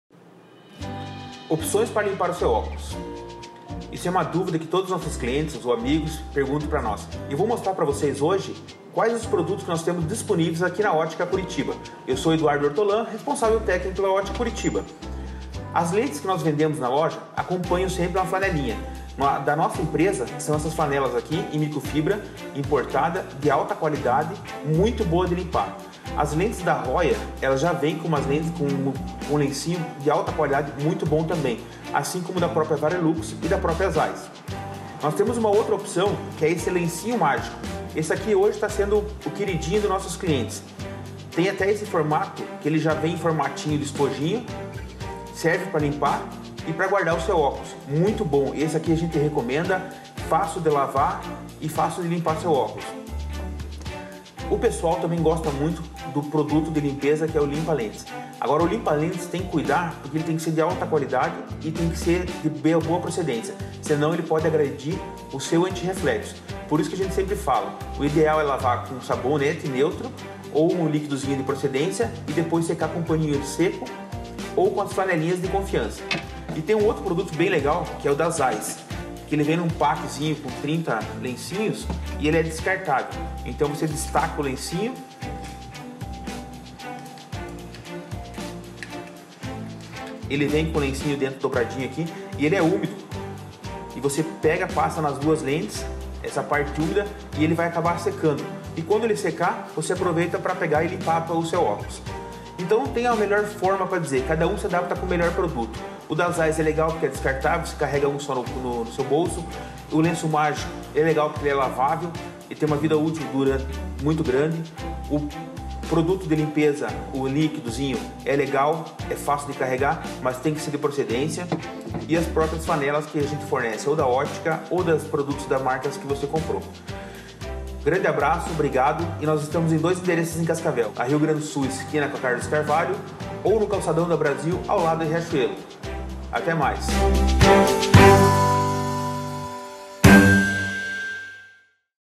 Flanelas para óculos - Ótica Curitiba
Como posso limpar meus óculos?

Bom cada um tem um método de limpeza mais eficaz, mas nesse vídeos vamos mostrar algumas soluções disponíveis aqui na Ótica Curitiba.

Estamos a disposição no WhatsApp pelos números:
📌Loja Rio Grande do Sul 928: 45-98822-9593
📌Loja Avenida Brasil 6436: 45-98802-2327

"Convênio com Estacionamento ao Lado do Banco Santander"